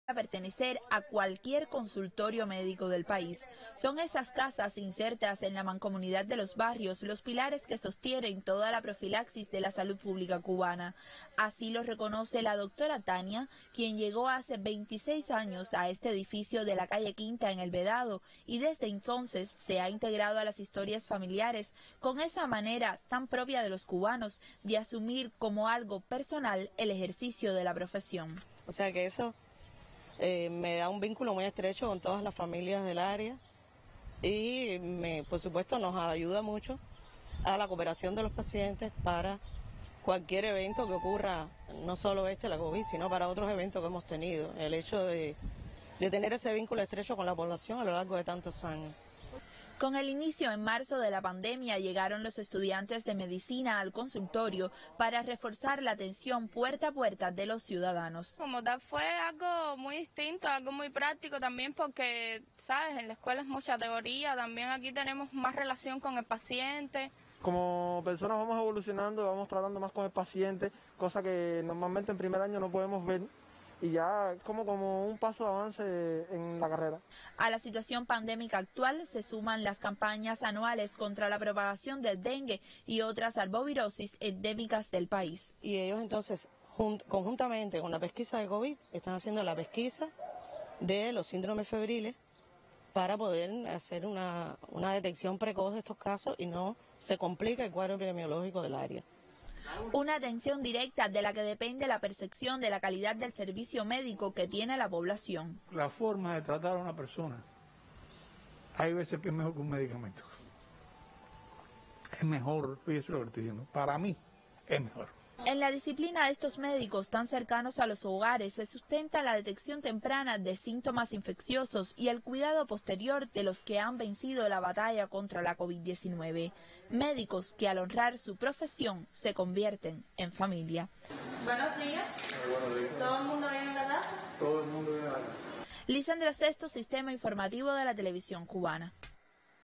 Temporada de lluvias en Cuba activa sistema de pesquisa para la prevención de enfermedades tropic
Con la llegada de la temporada de lluvias, el sistema de Salud nacional de Cuba suma a su labor la prevención contra el dengue y otras enfermedades tropicales. La pesquisa diaria que realizan miles de estudiantes y médicos por los barrios del país, incluirá la vigilancia ante los síntomas de las arbovirosis trasmitidas por el mosquito AedesAegypti.